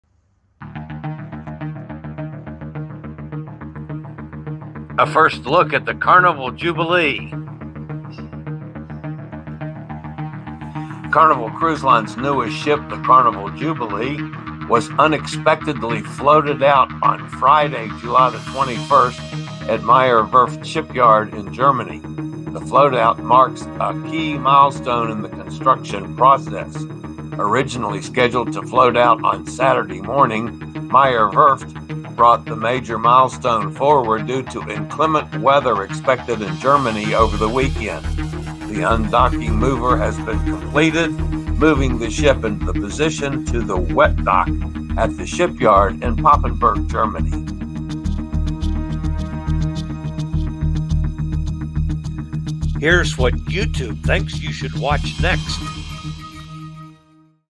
First Look At Carnival Jubilee
First Look At Carnival Jubilee Carnival Cruise Line’s newest Excel-class cruise ship has achieved another major milestone in its construction, touching water and floating for the first time since construction began in 2022. Meyer Werft stated that the float out, initially scheduled for Saturday, had been brought forward, likely due to the inclement weather conditions expected over the weekend.

NYTimes The Daily, the flagship NYT podcast with a massive audience. "Vacationing In The Time Of Covid"